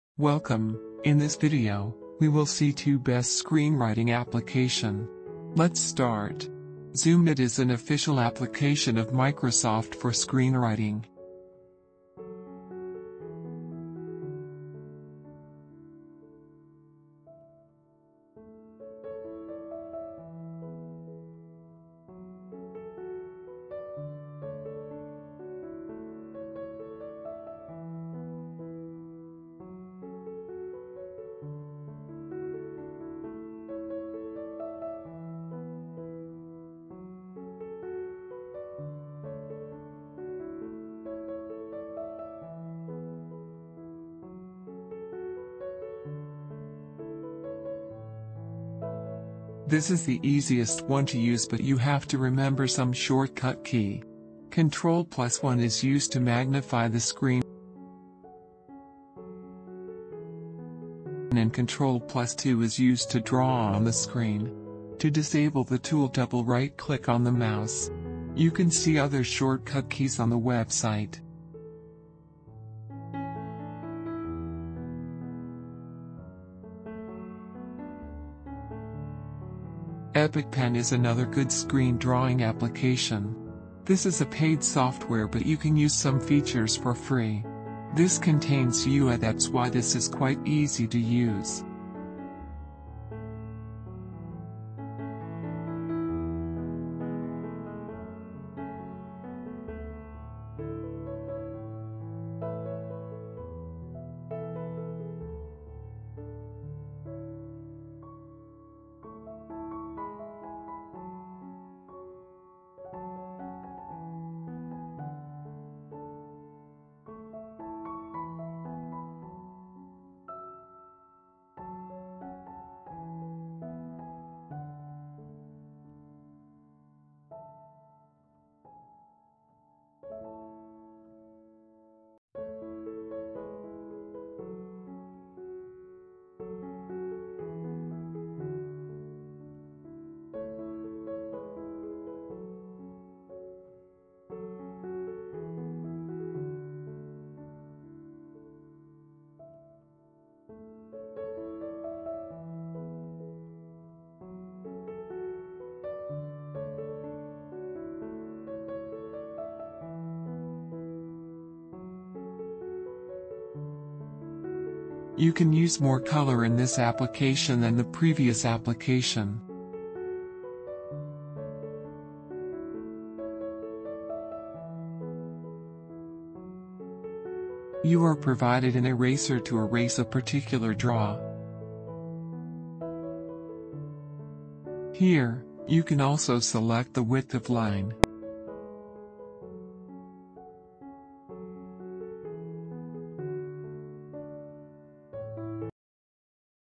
best free application to draw on a computer screen - How to draw on desktop
This video helps you to find the best drawing software on the screen of a laptop. The software discussed here is completely secure and free.